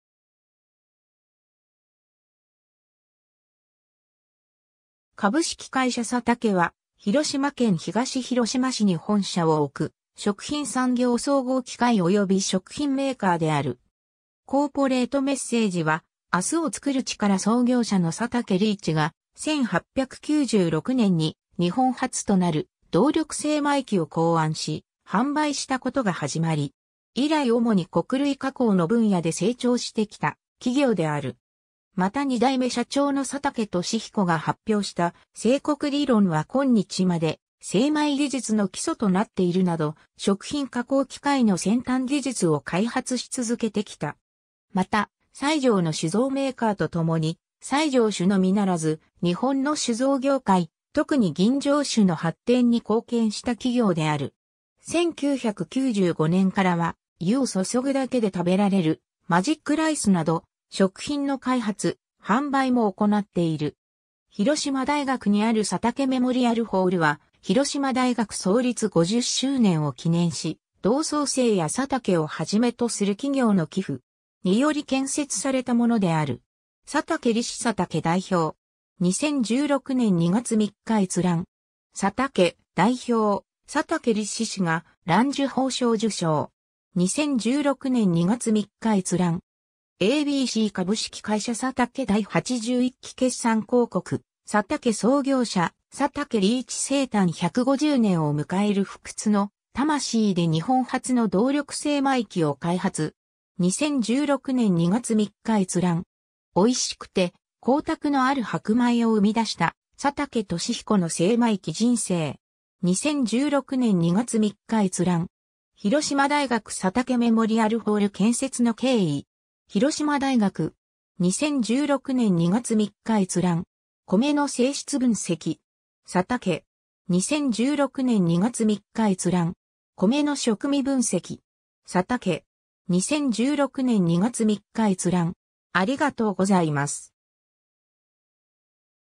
サタケ
株式会社サタケ（Satake Corporation）は、広島県東広島市に本社を置く、食品産業総合機械および食品メーカーである。
 コーポレート・メッセージは「明日を創る力」 創業者の佐竹利市が、1896年（明治29年）に日本初となる動力精米機を考案し、販売したことが始まり。
以来主に穀類加工の分野で成長してきた企業である。
また2代目社長の佐竹利彦が発表した精穀理論は今日まで精米技術の基礎となっているなど、食品加工機械の先端技術を開発し続けてきた。
また西条の酒造メーカーとともに西条酒のみならず日本の酒造業界、特に吟醸酒の発展に貢献した企業である。
 1995年（平成7年）からは、湯を注ぐだけで食べられる「マジックライス」など食品の開発、販売もおこなっている。
 広島大学にあるサタケメモリアルホールは、広島大学創立50周年を記念し、同窓生やサタケをはじめとする企業の寄付。
により建設されたものである。
 ^ “佐竹利子 サタケ代表”. 2016年2月3日閲覧。
 ^ “（株）サタケ　代表佐竹利子氏が藍綬褒章受章”. 2016年2月3日閲覧。
 ^ a b c 株式会社サタケ 第81期決算公告 ^ “サタケ創業者 佐竹利市生誕150年を迎える--- 不屈の魂で日本初の動力精米機を開発 ---”. 2016年2月3日閲覧。
 ^ “おいしくて光沢のある白米を生み出した佐竹利彦の精米機人生”. 2016年2月3日閲覧。
 ^ “広島大学サタケメモリアルホール建設の経緯”. 広島大学. 2016年2月3日閲覧。
 ^ “米の性質分析”. サタケ. 2016年2月3日閲覧。
 ^ “米の食味分析”. サタケ. 2016年2月3日閲覧。